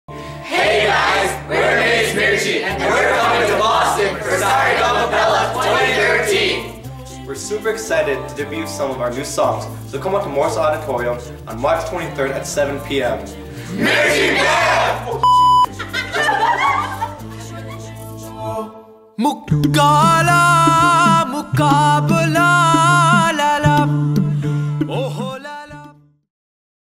Sa Re Ga Ma Pella 2013 - Maize Mirchi!
Sa Re Ga Ma Pella 2013 - Here's a message from one of the performing groups! Get excited for March 23rd!